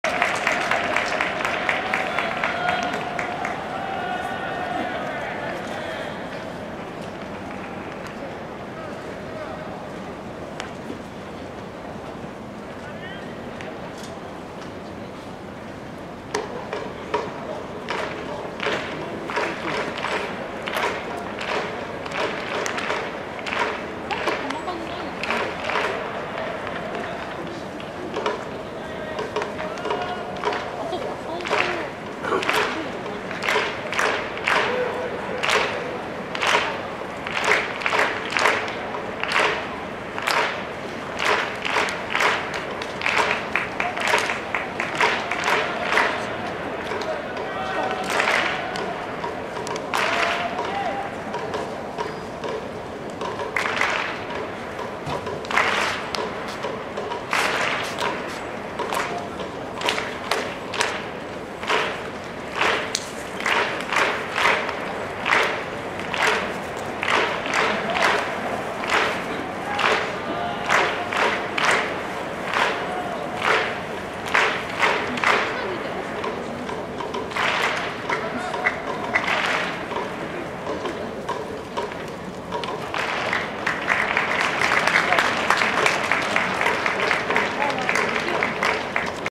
山川穂高 変化球を見極め四球を奪う 【読売ジャイアンツ対 西武ライオンズ2022年3月2日 東京ドーム】
この試合 下記再生リストにまとめました
読売ジャイアンツ対 西武ライオンズ2022年3月2日 東京ドームオープン戦